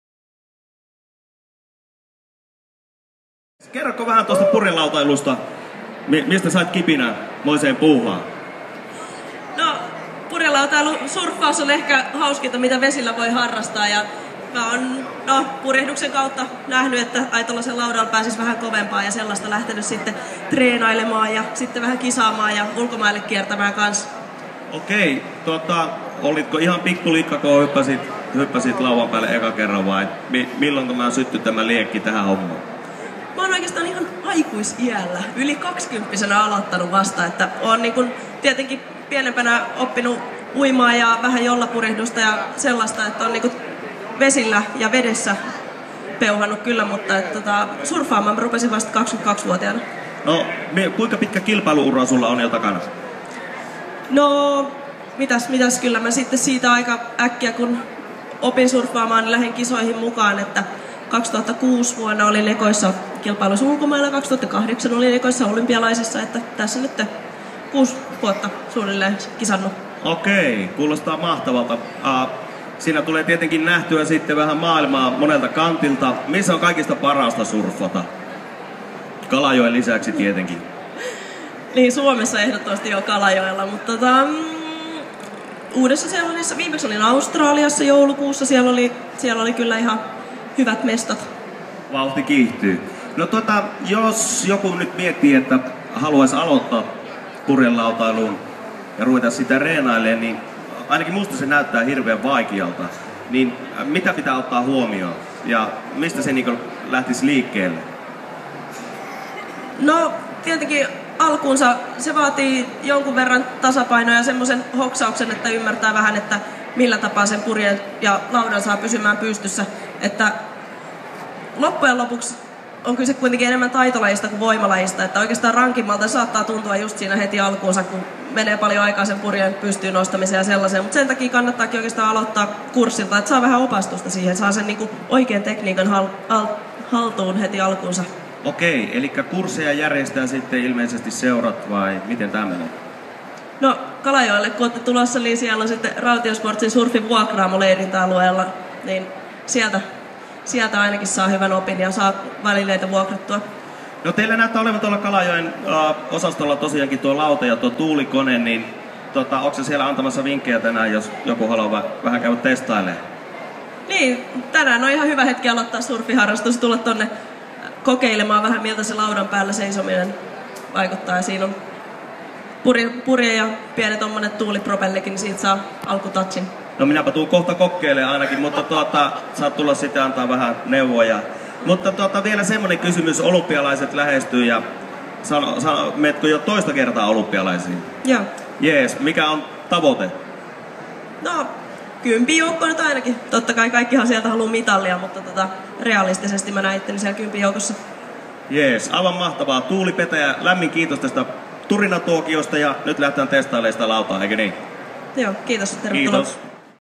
Tuuli Petäjä / Matkamessut
Tuuli Petäjän haastattelu matkamessuilta. Mukana Rautio Sports Surf Center/ Kalajoen osasto 6m99